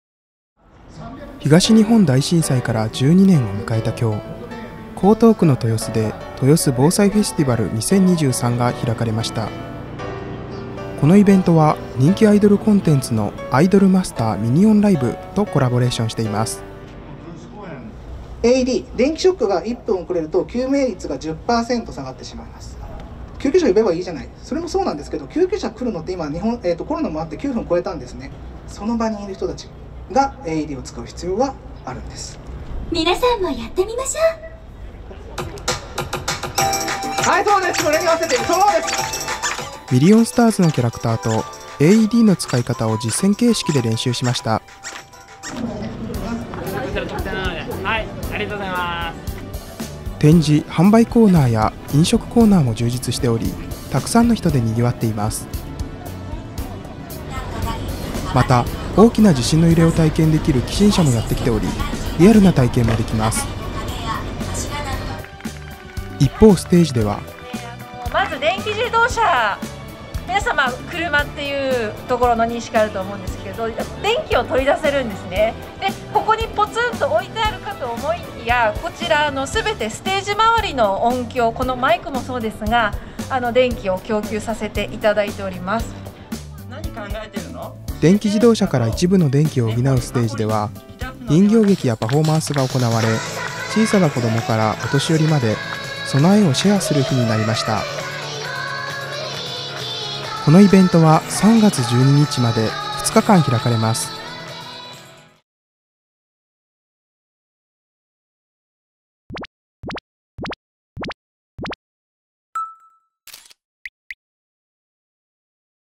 《NEWS》豊洲ぼうさいFestiv@l 2023【XF605】
ST-1とSBM-01のテストを兼ねて「豊洲ぼうさいFestiv@l 2023」の撮影をしました。
手持ちにしたり三脚に載せたりと何度も三脚から外すため、すぐにバランスが取れた状態になるのでよかったが、重すぎてNH10ではギリギリカウンターバランスが効かなくなった…

・日時 / 2023年3月11日(土)～12日(日) 10:00～17:00
・場所 / 豊洲公園 … 東京都東区豊江洲 2-1　豊洲公園 花木とモニュメント広場
　　　　 そなエリア東京 … 東京都有明3丁目8-35　東京臨海広域防災公園
　　　　 がすてなーに … 東京都江東区豊洲6丁目1-1
・主催 / 江東区の地域防災力向上を目指す会
　　　　 ぼうさいFestiv@l事務局

【撮影機材】
・XF605（カメラ）
・NH10（三脚ヘッド）
・ECM-VG1（マイク）
・ST-1（三脚アダプタープレート）
・SBM-01（三脚ベース）

【撮影日】
2023年3月11日

【音楽】
DOVA-SYNDROME